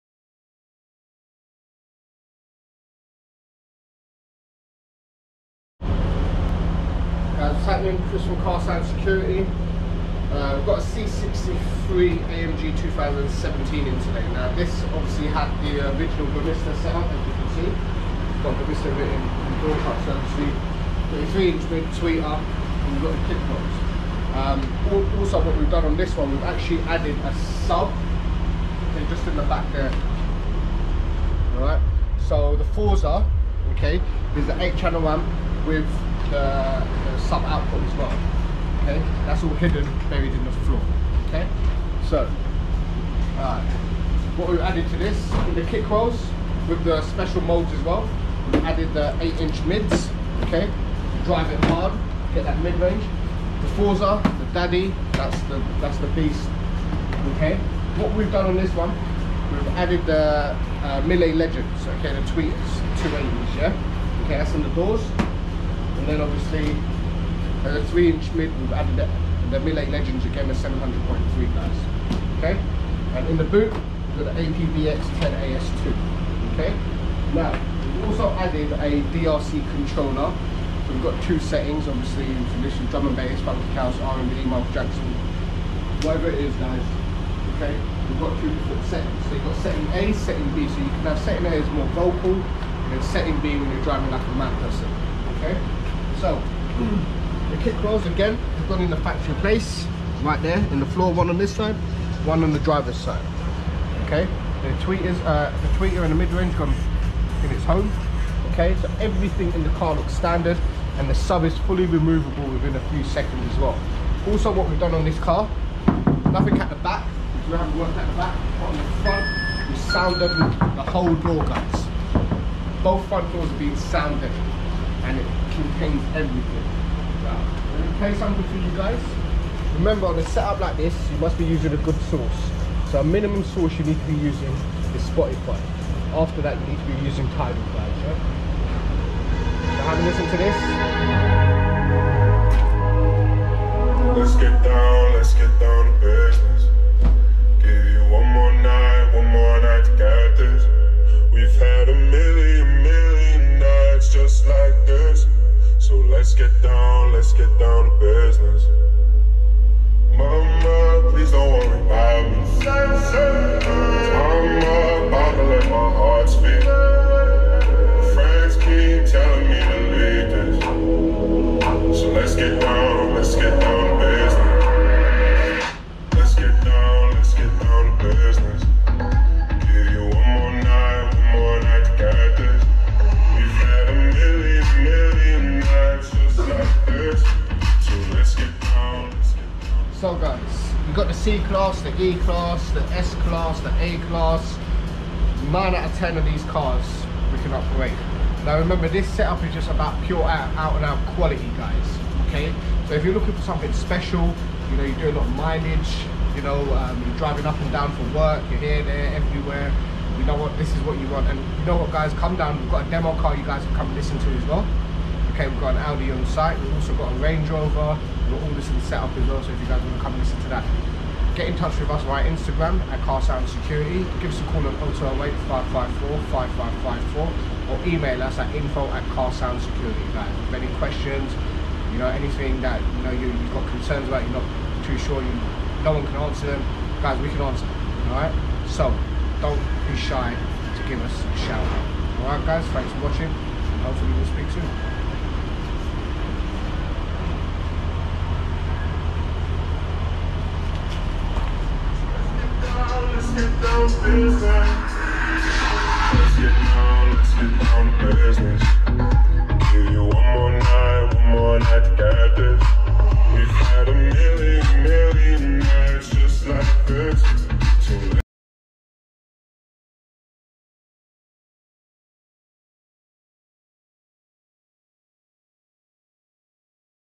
Mercedes C Class C63 AMG 2017 Audison Sound System Upgrade With Hertz And DRC MP Controller
We have upgraded this Mercedes C63 AMG Factory audio system with a more high quality and hi-res sound system that blows the factory audio away. We have Installed a full audison sound upgrade with hertz. This system gives you an amazing sound listening experience with rich clarity and nice sounding bass.

Store Number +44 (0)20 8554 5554